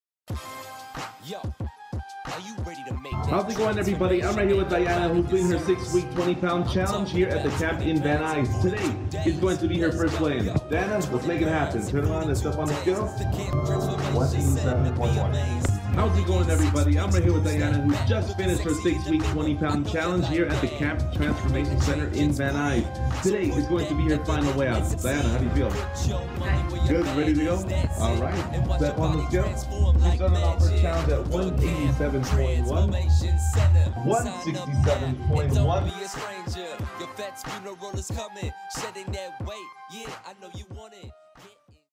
Van Nuys Fitness 6 Week Challenge Results -Diana Stephens
We will now check the result of Diana's 6 week weight loss challenge!